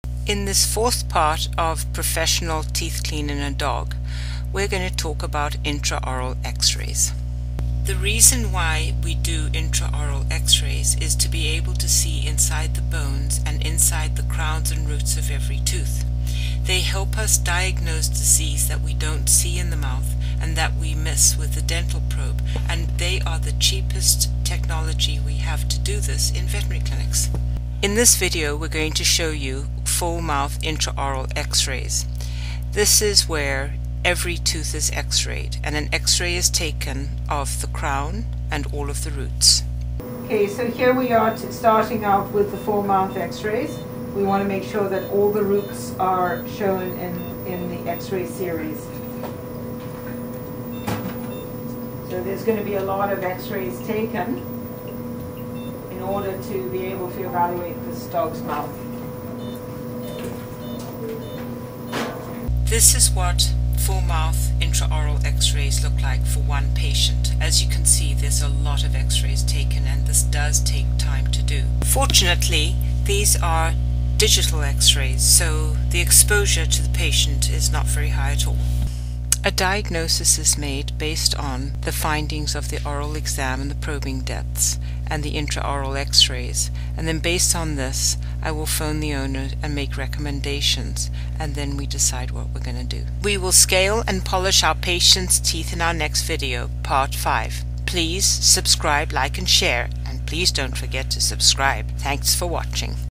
Professional Teeth Clean in a Dog: Part 4 - Full Mouth X-rays
How can your veterinarian diagnose what is happening in the bone below the gumline? X-rays are a wonderful diagnostic helper so that  we can actually see what is going on around the roots of the teeth. Find out what full mouth X-rays are and why they are recommended by veterinarians.